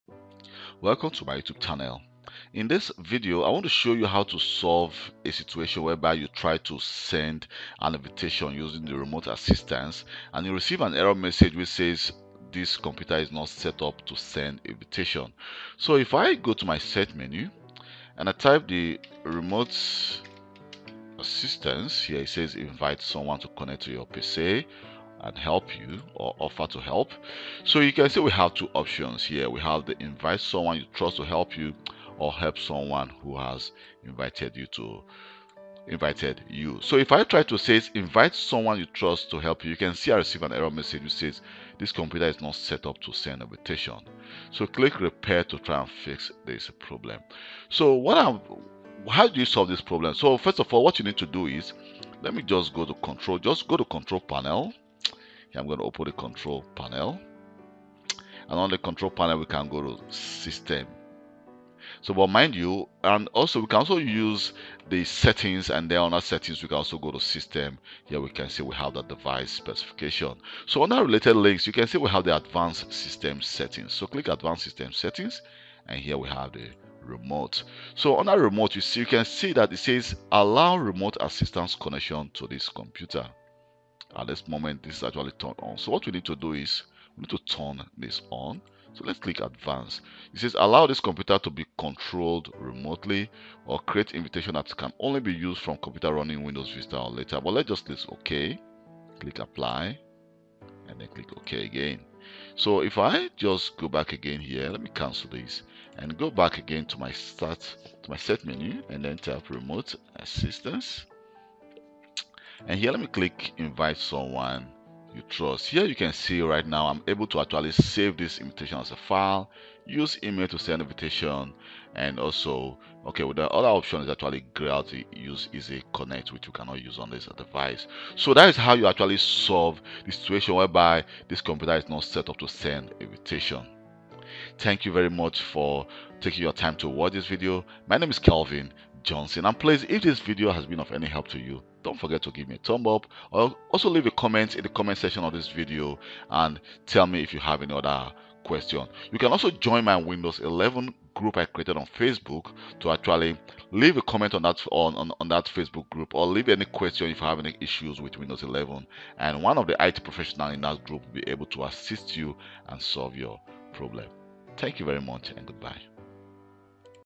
Solved - This computer is not set up to send invitation | Fix Windows Remote Assistance Windows 11
Solved - This computer is not set up to send invitation - Fix Windows Remote Assistance issue.

Looking to elevate your IT skills to the next level? Check out this amazing course on Mastering IT Systems Administration & Azure Cloud Engineering by Udemy!